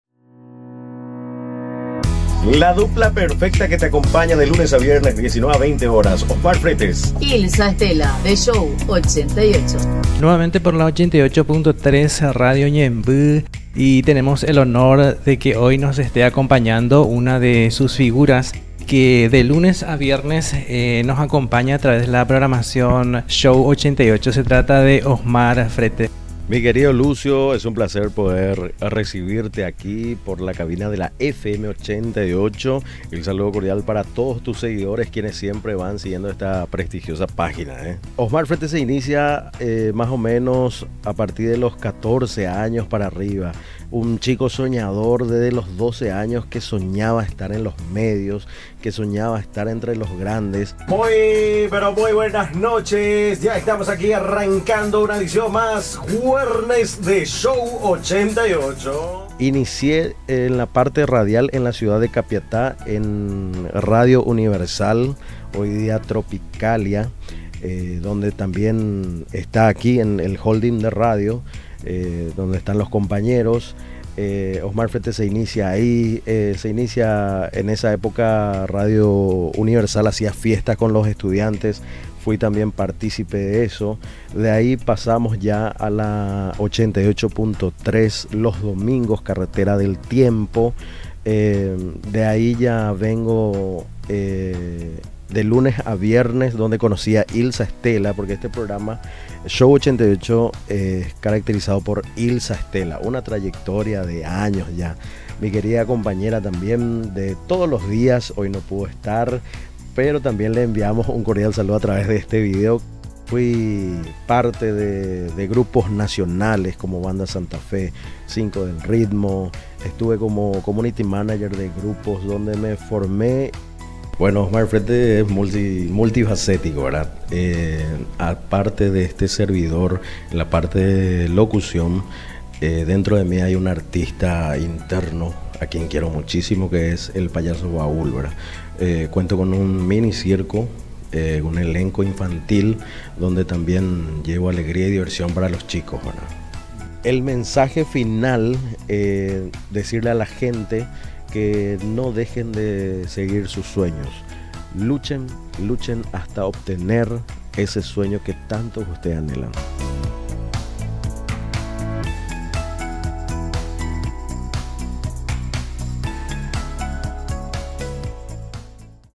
Show 88 - Radio Ñemby
Compartimos una amena charla con Osmar Fretes quien nos cuenta sobre sus inicios y trayectoria en los medios de comunicación. Actualmente está acompañando en la conducción del programa Show88 por Radio Ñemby 88.3 Fm, de lunes a viernes. A partir de las 19 horas.